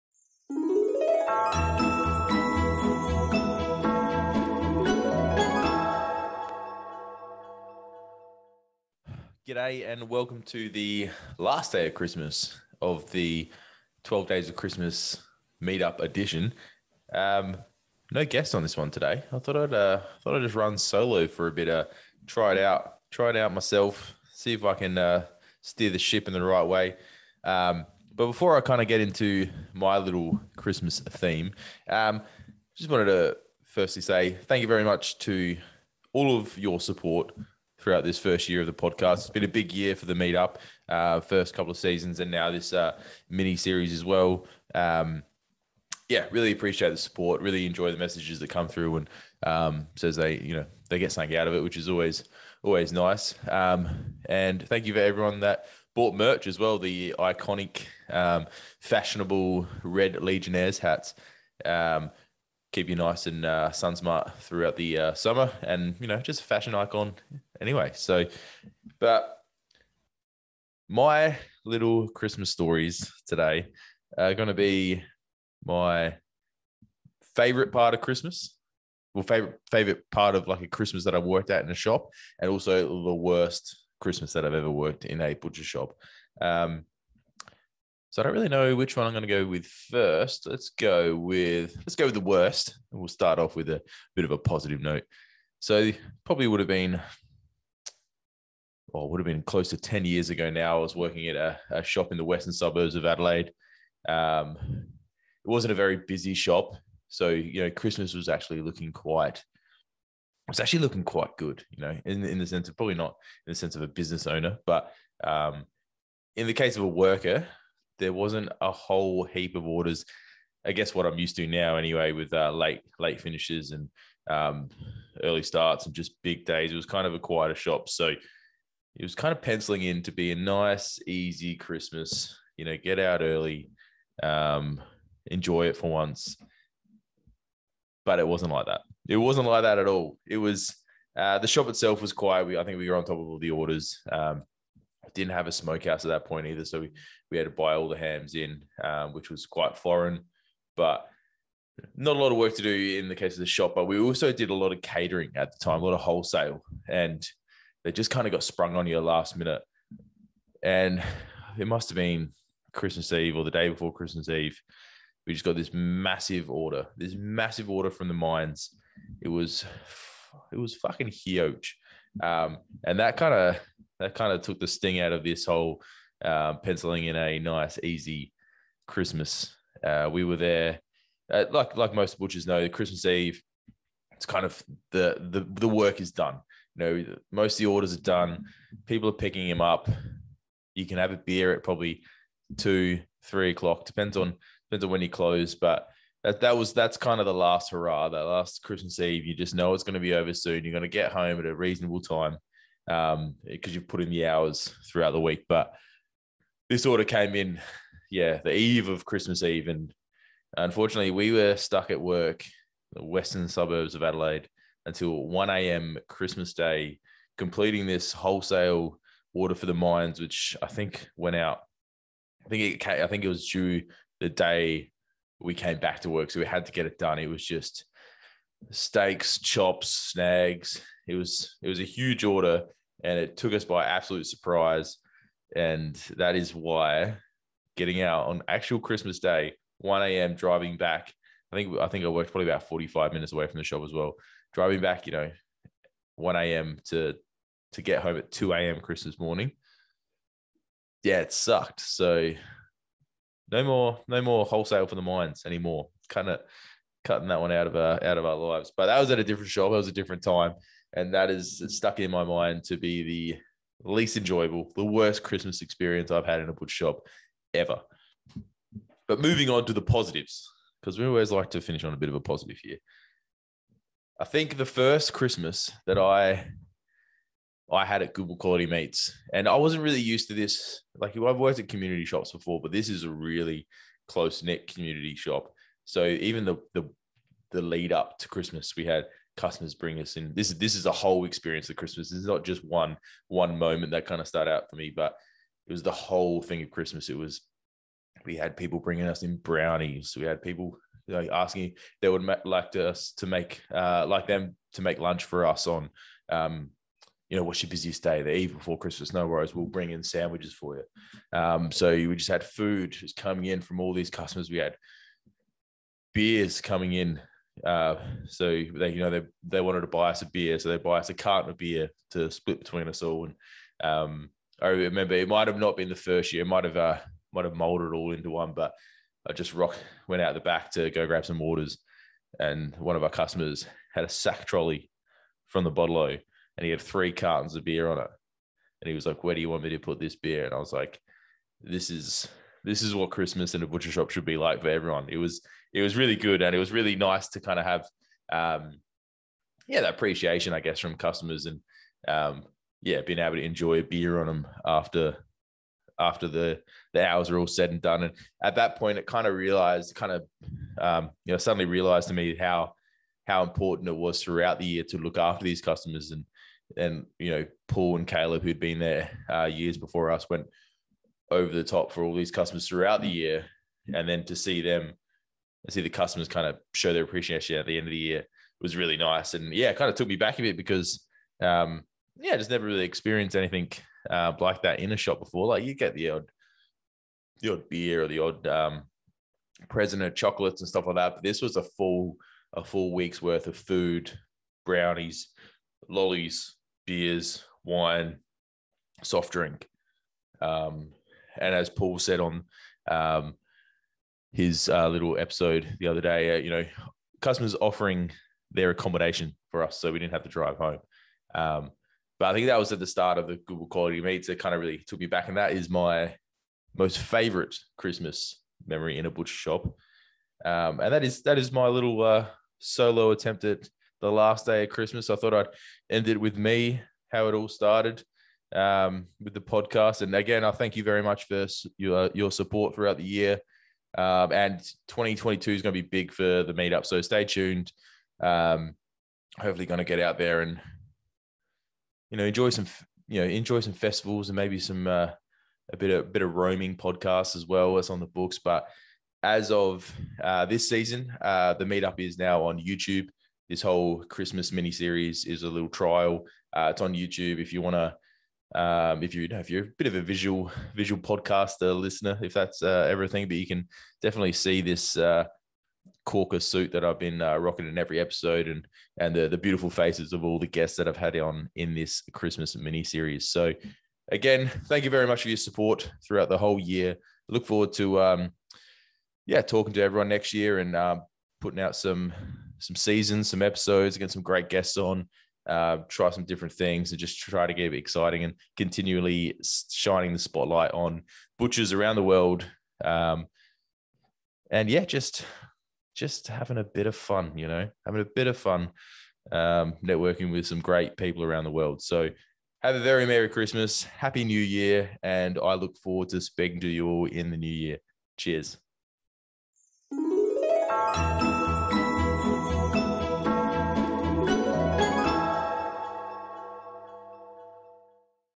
Luke Leyson | 12th Day of Christmas | The Meat Up Podcast
On the twelfth day of Christmas the Meat Up brought to you, Luke Leyson! I thought I'd go solo and share with you my best Christmas as a butcher and my worst.

That's a wrap for the 12 days of Christmas mini series I hope you enjoyed it!